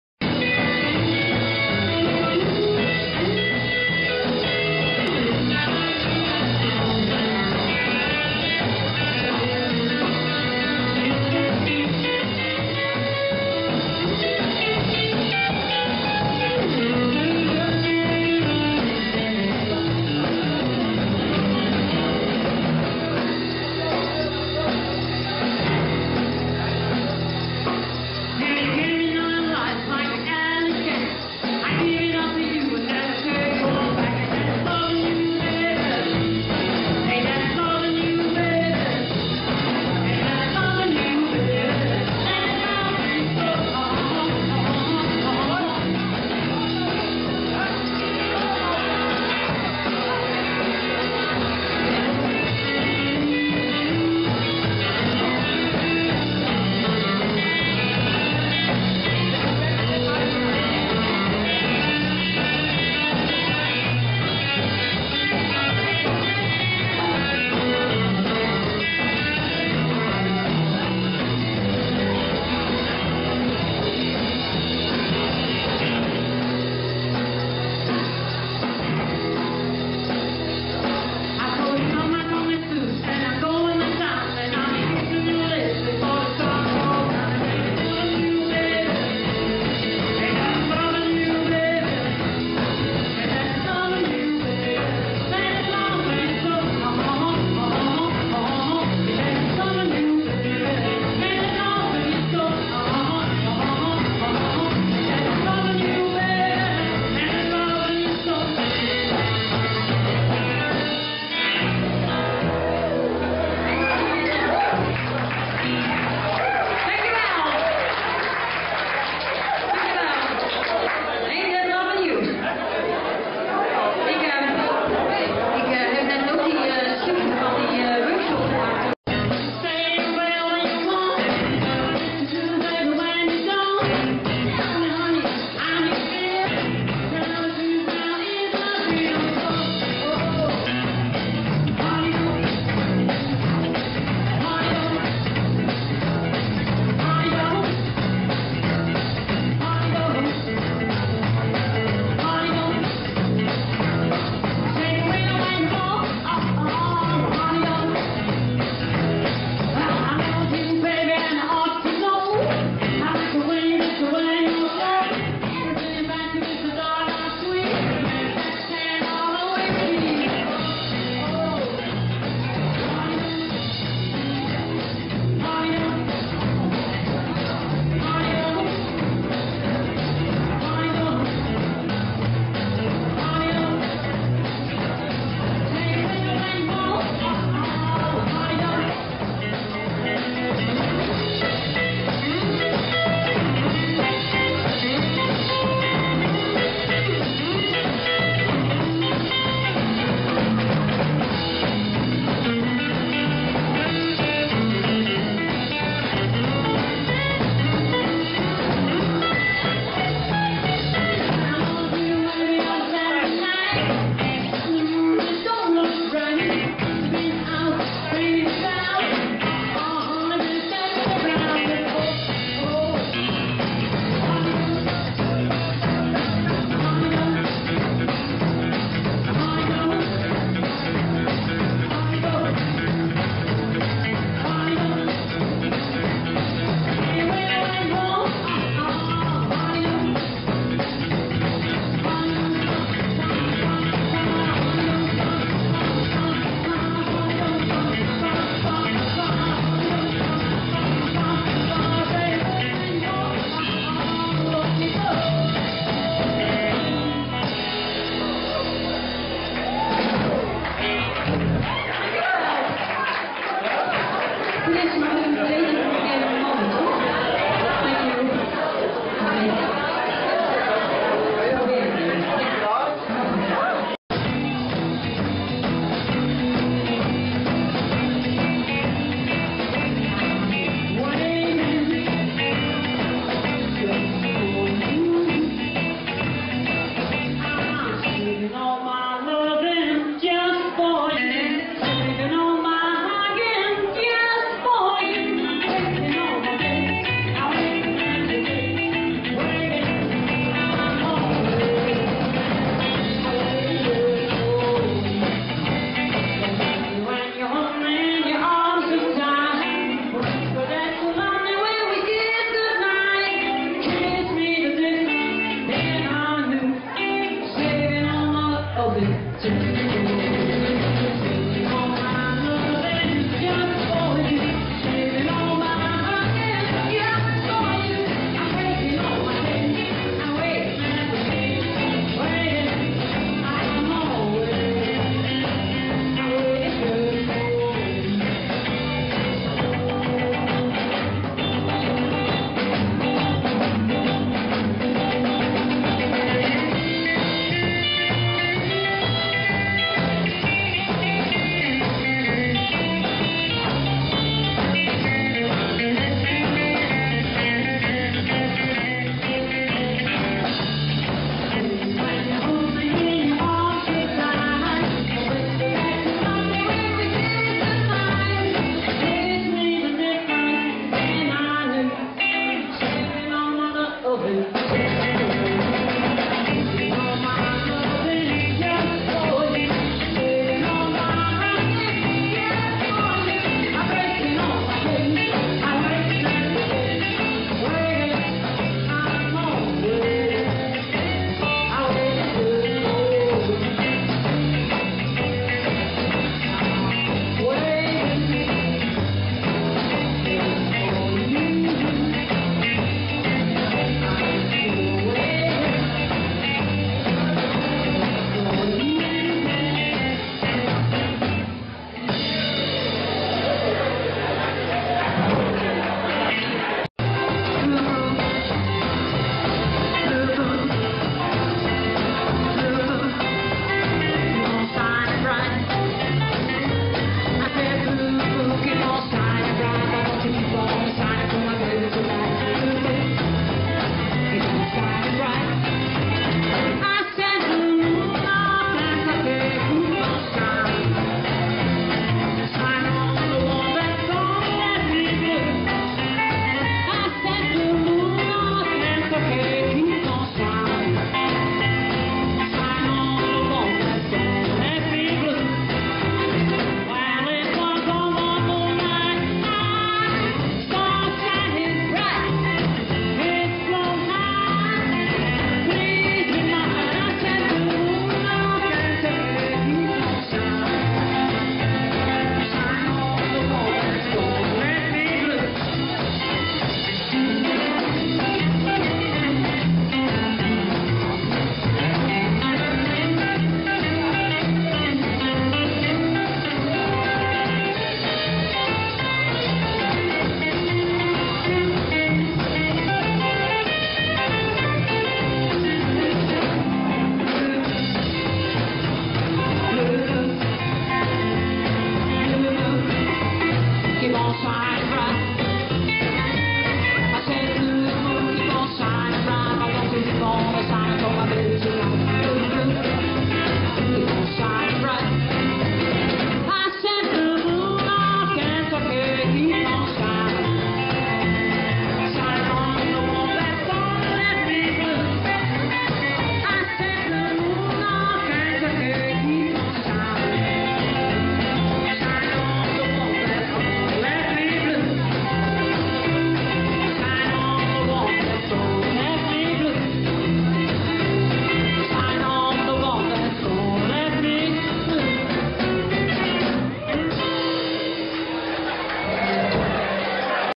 End of year party
Een gezellige dansavond van Let's Swing in Groningen
Deel 1